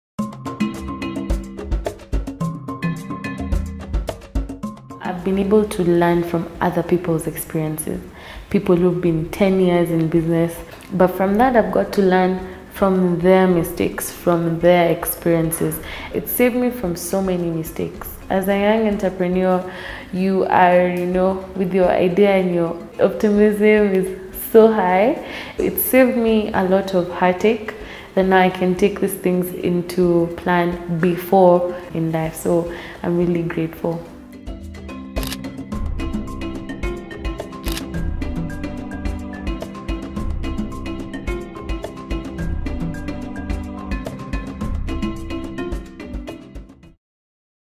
Sinapis Testimonial // Keisla //Learning from others
Sinapis alumna Keisla shares how valuable it was for her to be able to learn from the experiences of her fellow classmates who had been in business longer.  Their entrepreneurial journey helped her set up her business for success.